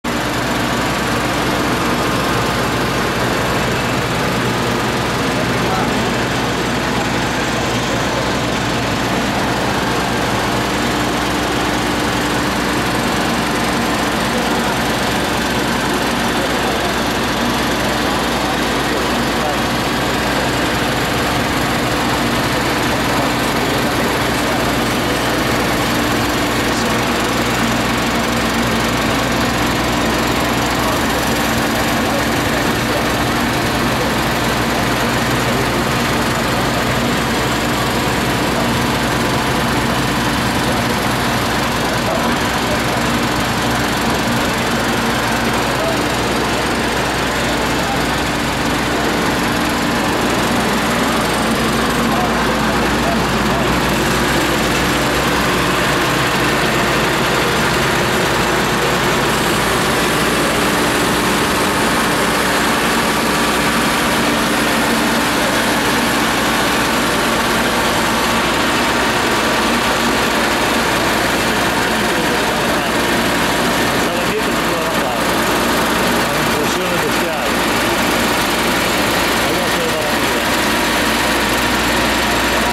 CLAAS TUCANO 430 MONTANA 4 si sgranchisce.
Un breve video della nostra TUCANO MONTANA 4 durante una presentazione ai nostri concessionari la scorsa estate. Tempo di fare un po' di stretching!